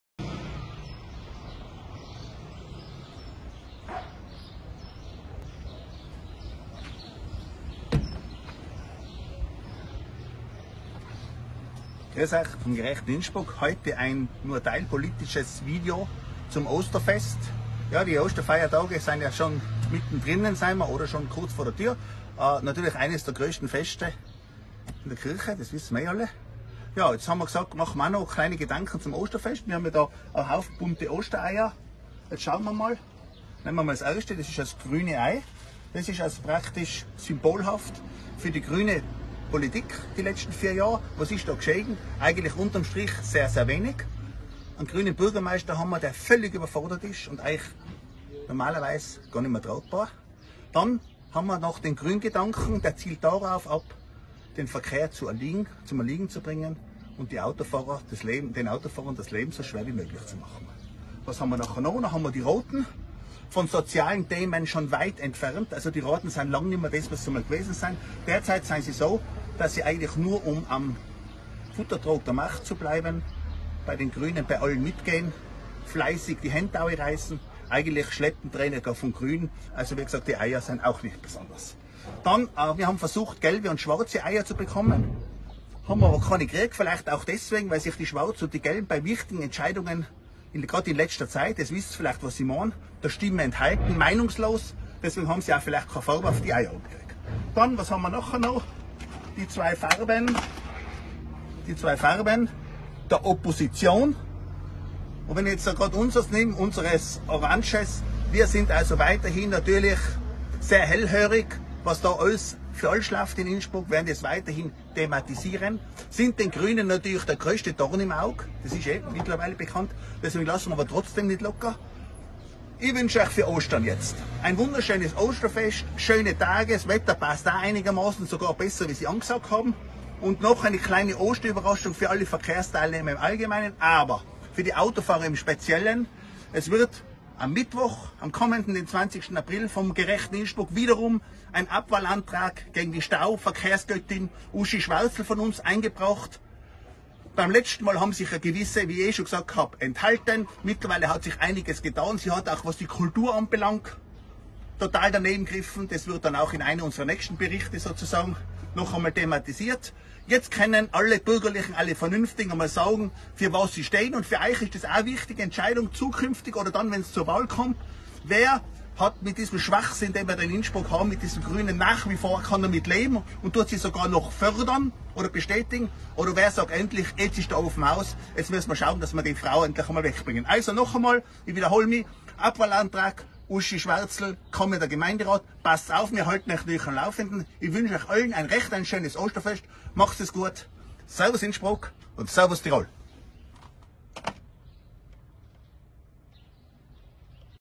Frohe Ostern 2022 - Vorschau Uschis Abwahl
Unser OSTERVIDEO mit kleinem Rückblick über die bisher gelegten
,,faulen Eier" der Grünfraktion, und Vorausblick auf den kommenden
Gemeinderat am MITTWOCH 20 April 2022 .
FROHE OSTERN allen Innsbruckern allen Mithelfern für ein
GERECHTERES INNSBRUCK